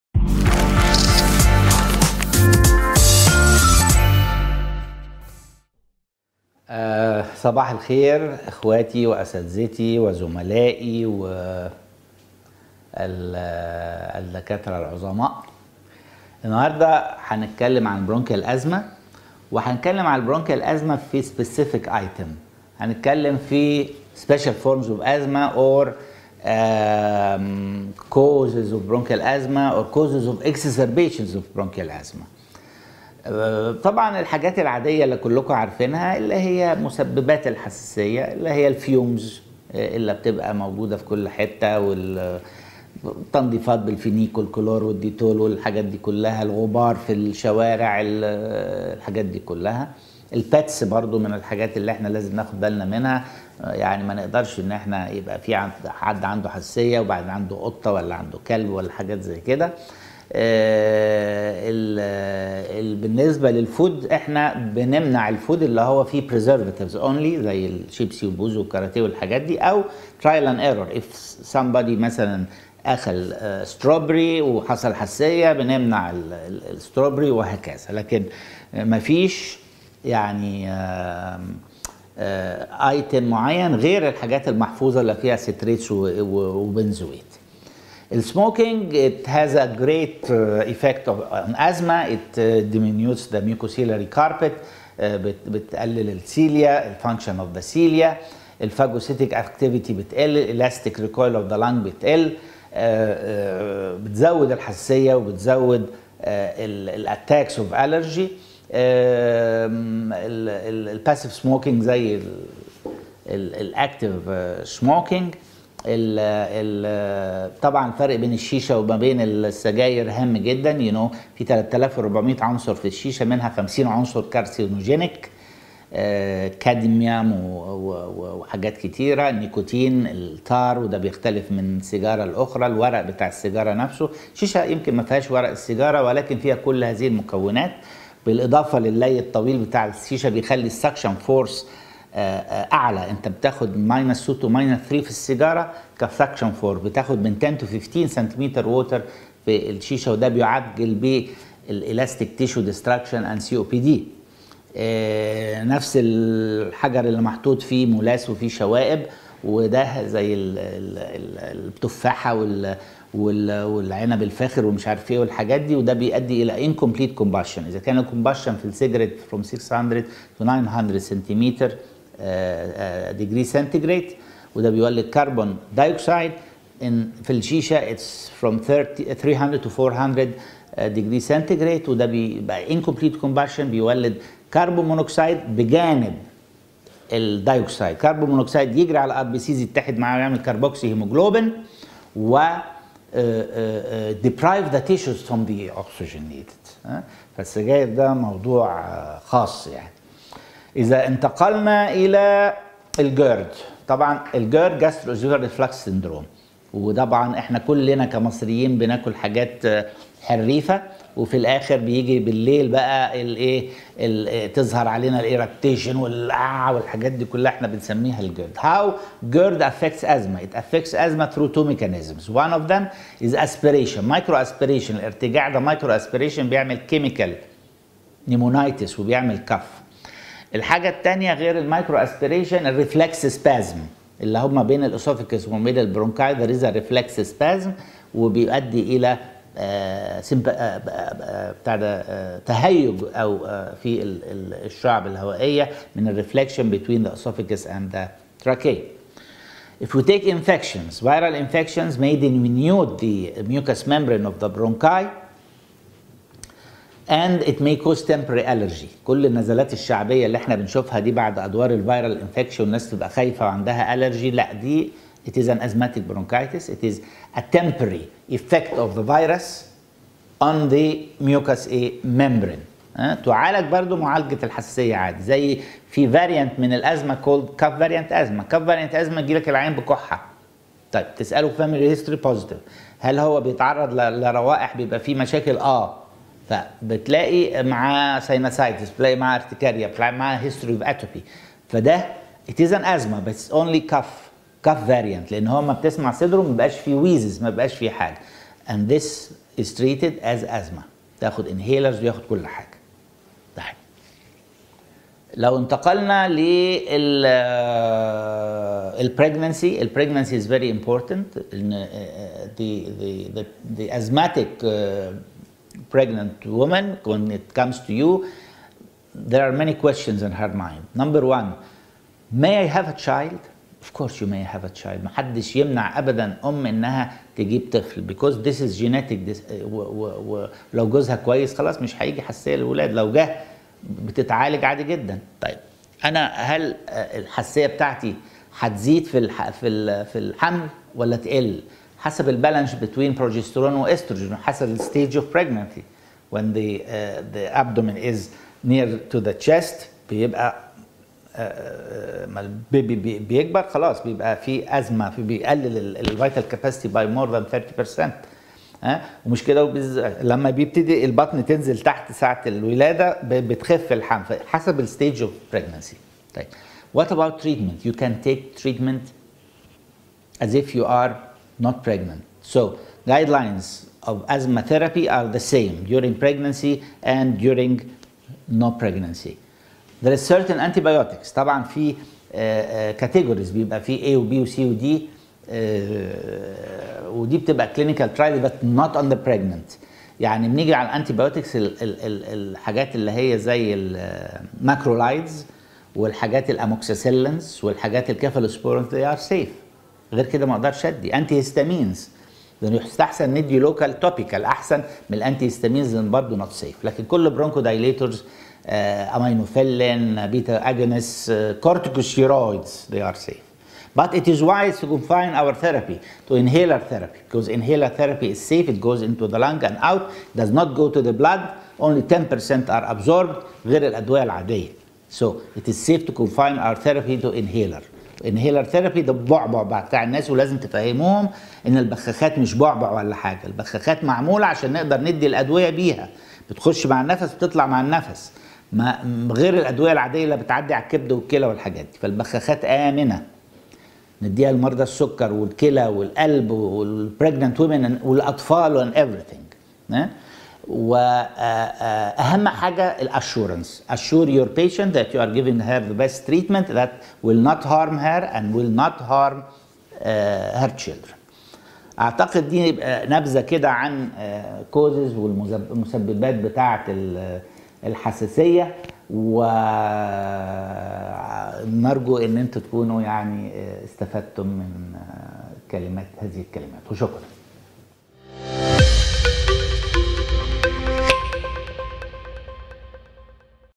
Prof. Essam Gouda talks about proncal asthma
Prof. Essam Gouda
Professor of chest diseases and allergy, Faculty of Medicine, Alexandria University